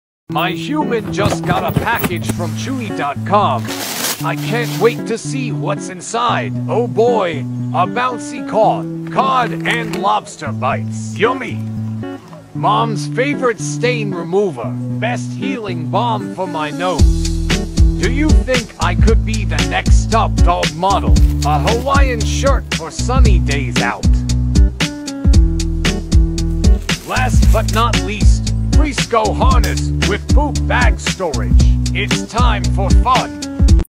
Unboxing chewy goodies
🐾🛍️ A pup's dream come true: unboxing goodies! Toys, treats, and more! 🐕✨